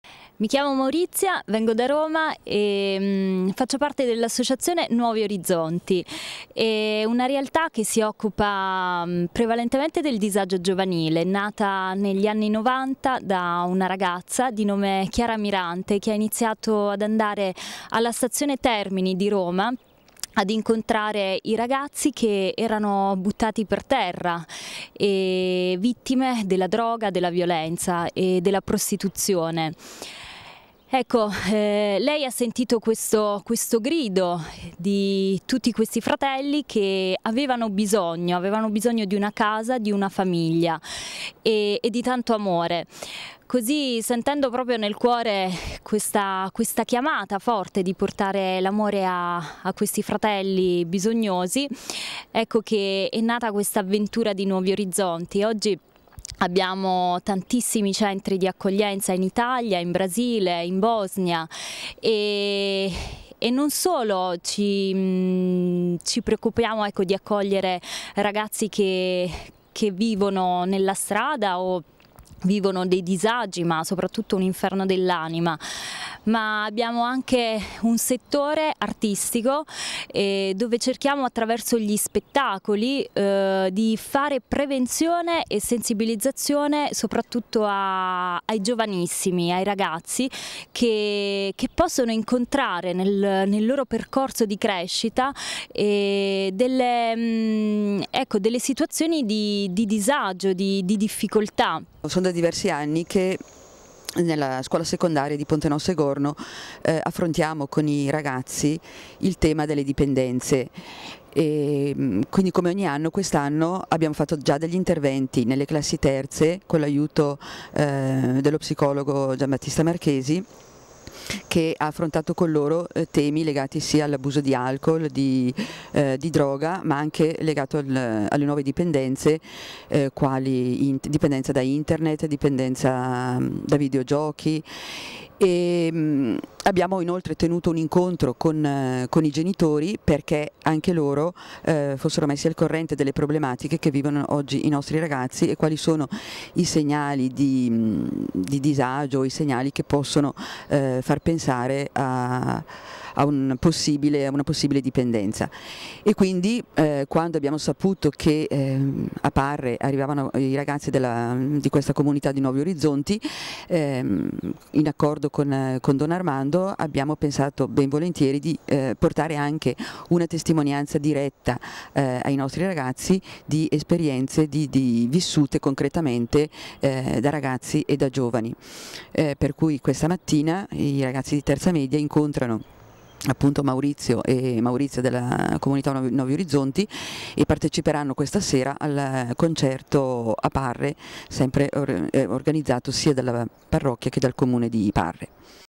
Ponte Nossa ospita comunità Nuovi orizzonti Antenna 2 TV 290311.mpg
Sabato (26 marzo 2011) alle scuole di Ponte Nossa abbiamo incontrato alcuni rappresentati della comunità nuovi orizzonti. Ve la presentiamo.